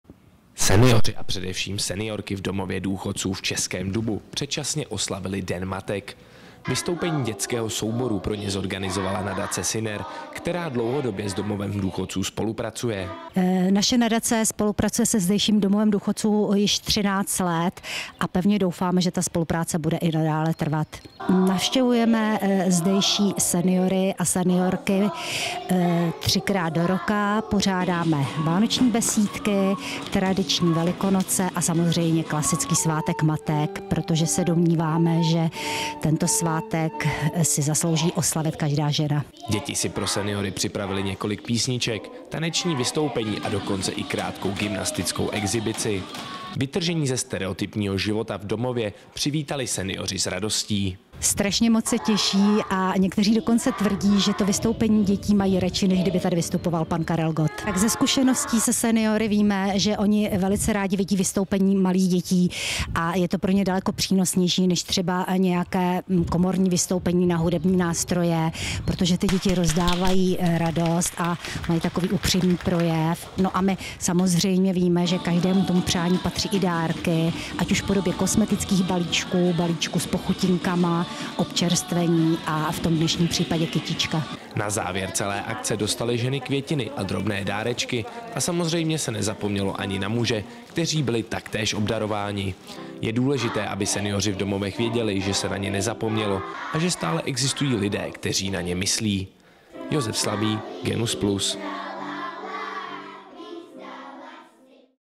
Den matek v domově důchodců v Českém Dubu
Předčasně oslavili den matek v domově důchodců v Českém Dubu. Nadace Syner zorganizovala vystoupení dětského souboru pro seniory a seniorky.